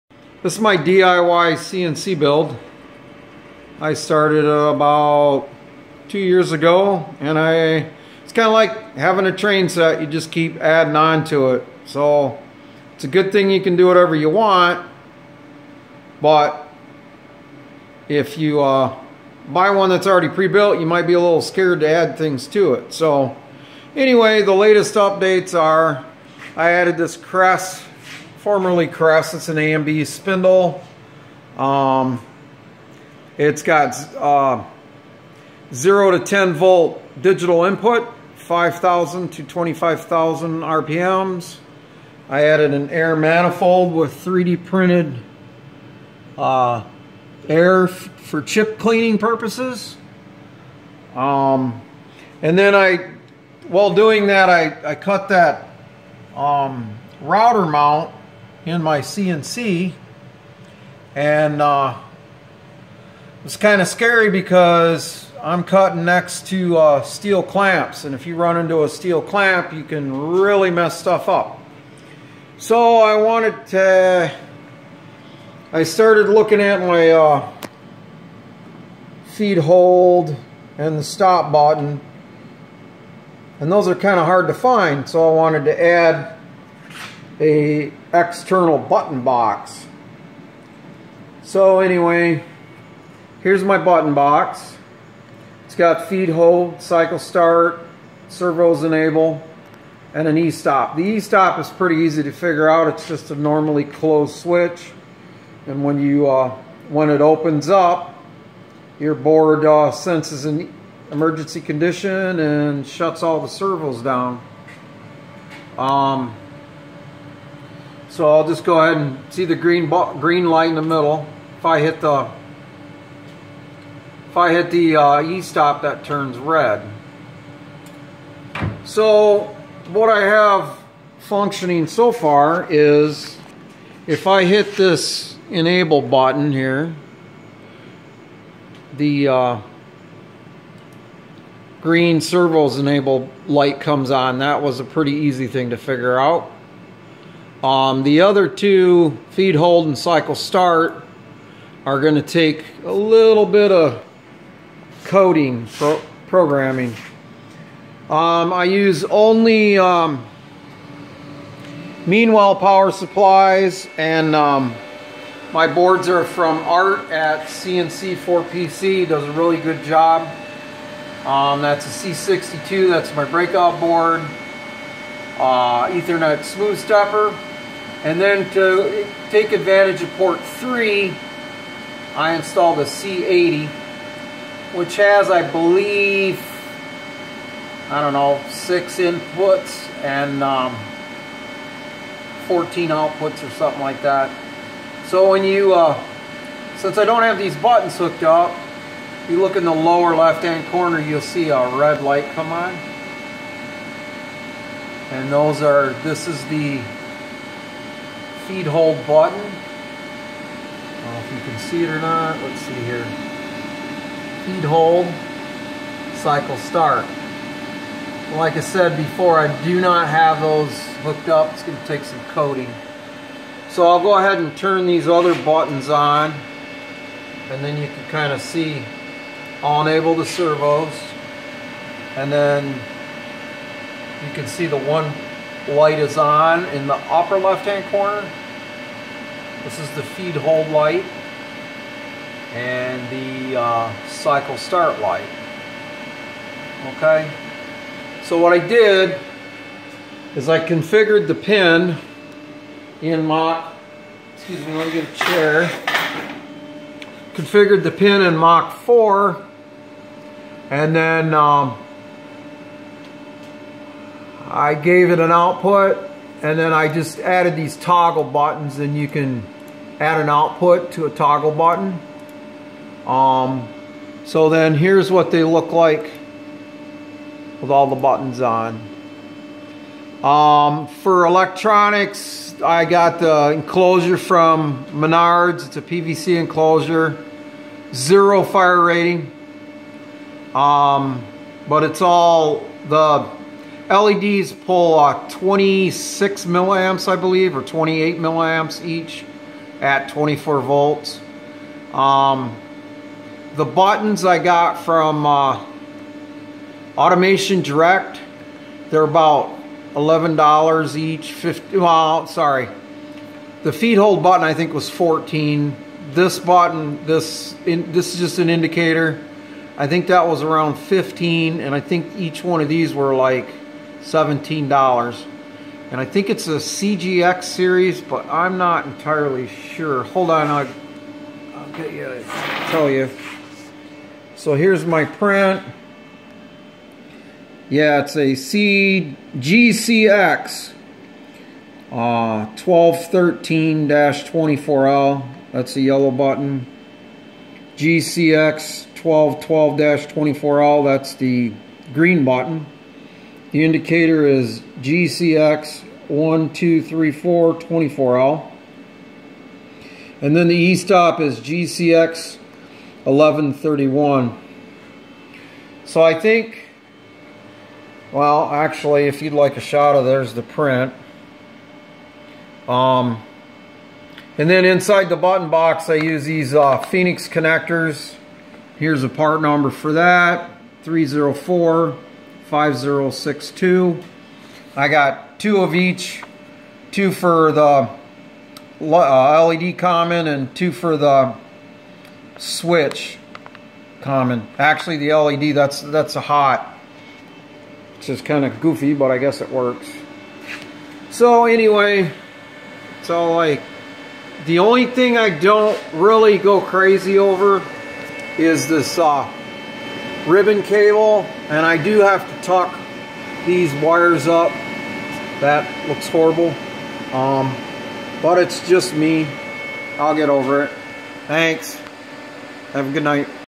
DIY CNC updates.
Spindle/router update, lighting addition, and button box addition.